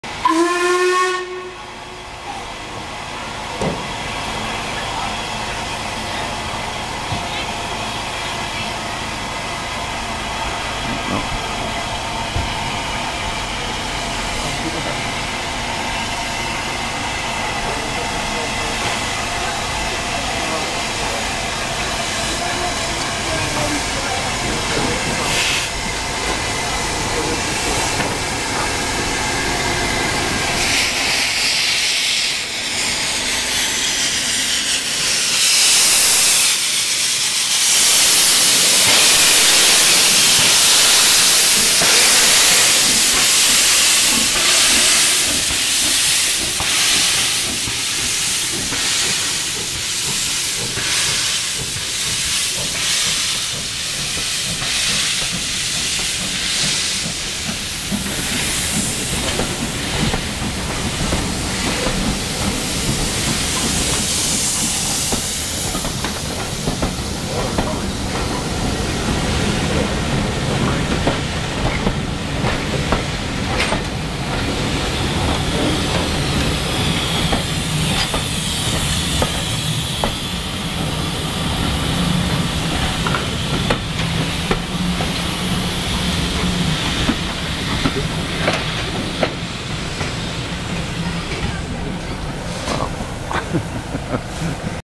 Steam Train of the NYMR getting a boost start
A locomotive of the North Yorkshire Moors Railway getting a helping hand from a pusher steam locomotive. The steam runs are regularly scheduled from March to November and are a real hoot. Stations are very old - one was used for Harry Potter films I believe.